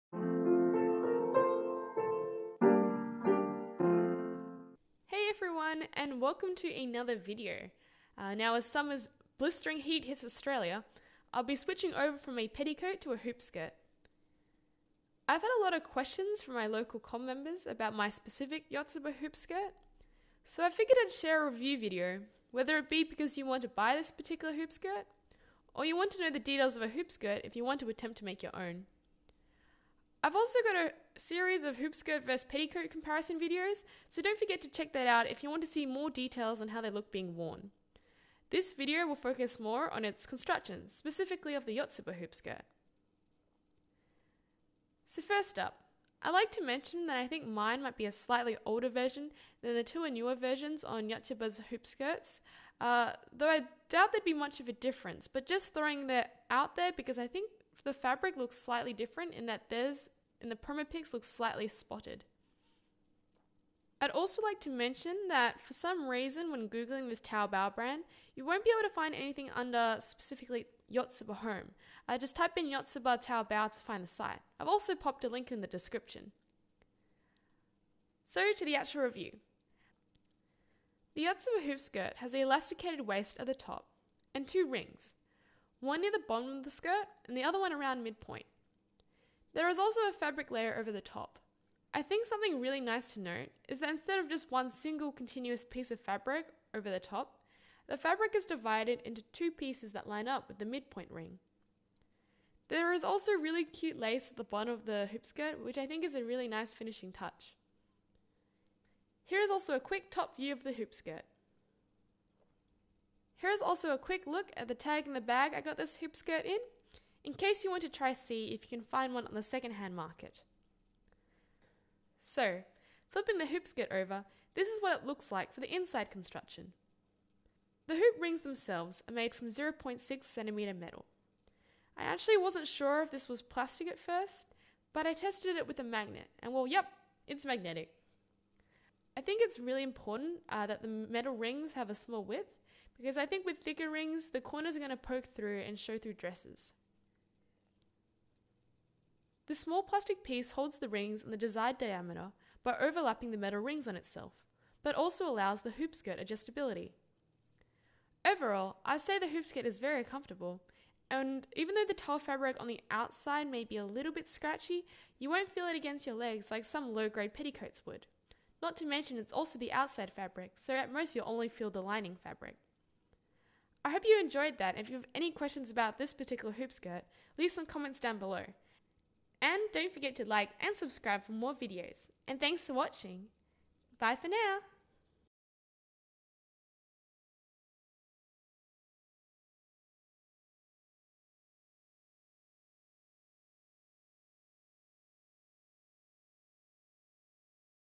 Hoop Skirt Review Video - Yotsuba Home hoop skirt
Link to taobao Yotsuba (You'll need a chinese shopping service to forward it to you :) )
I figured I'd do a proper review video for this hoop skirt in case anyone was looking to buy one /wanted to make one and wanted to have an idea of construction.

Enjoy the video and sorry I haven't been uploading videos in a while. I've been so busy !!